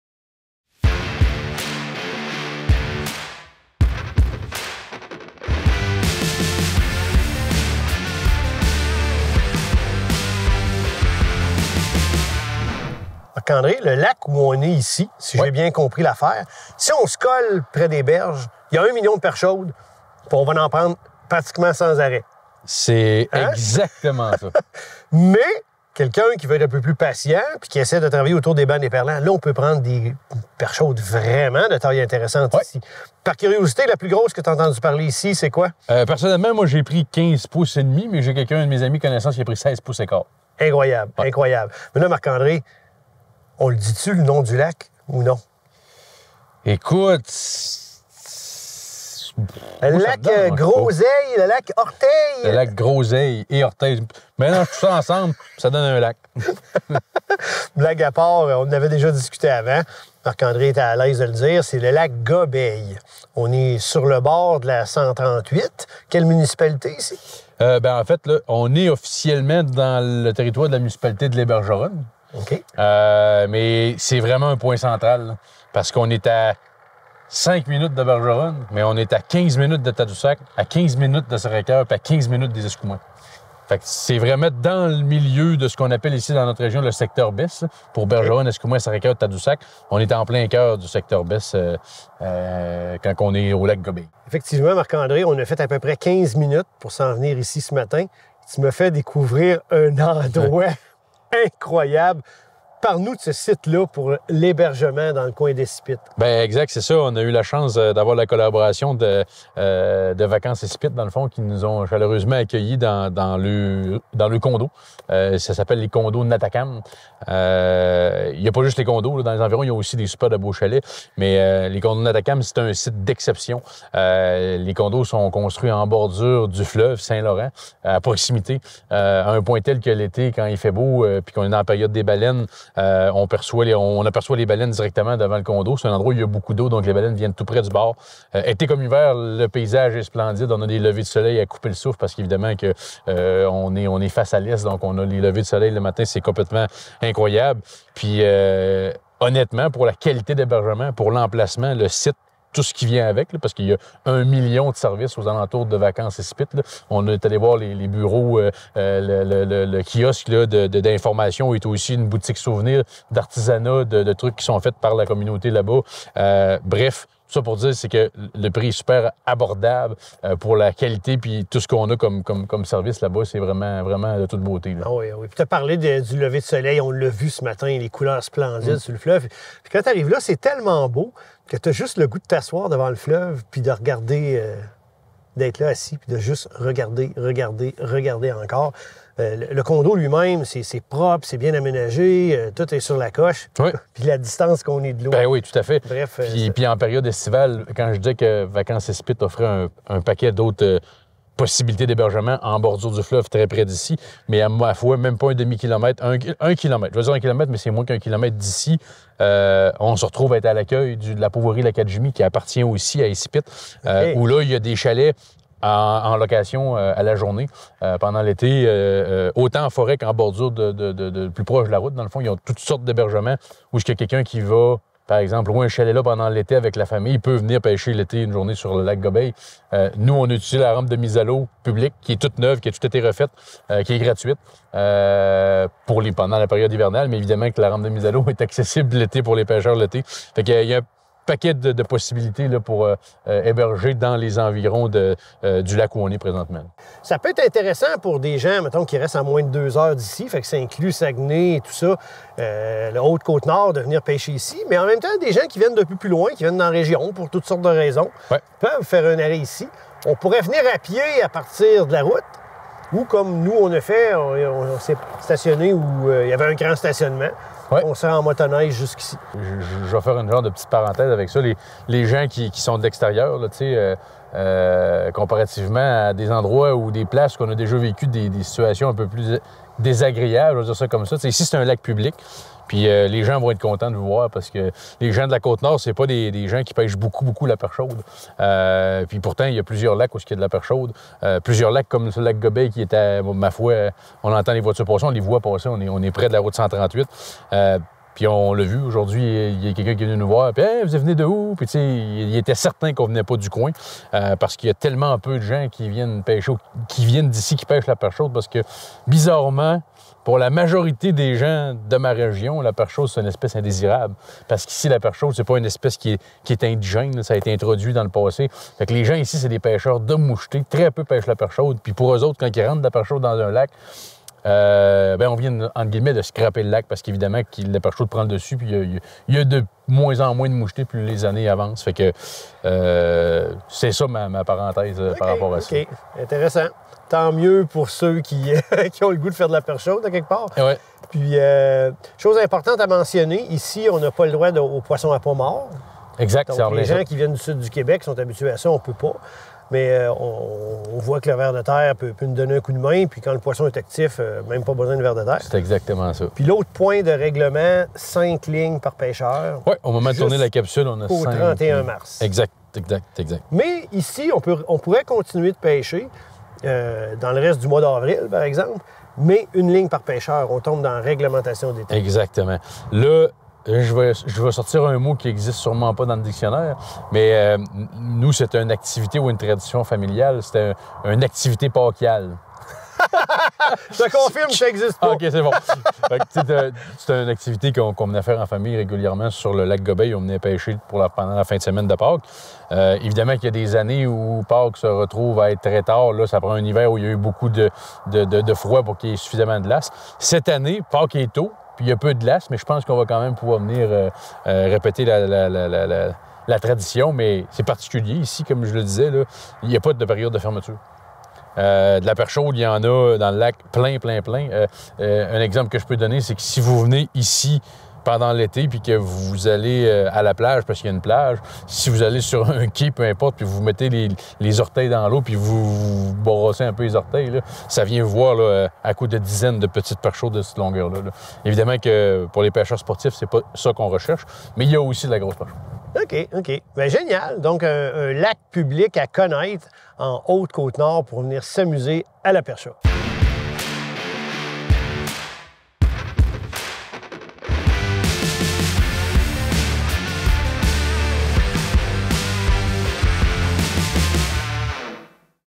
Capsule Destination - Pêche à la perchaude - Côte-Nord
Voici une présentation du secteur de pêche de l'émission de la semaine, la magnifique Côte-Nord ainsi qu'un endroit de choix pour vous loger confortablement devant une vue imprennable sur le Fleuve Saint-Laurent et un endroit formidable pour la pêche de plusieurs espèces.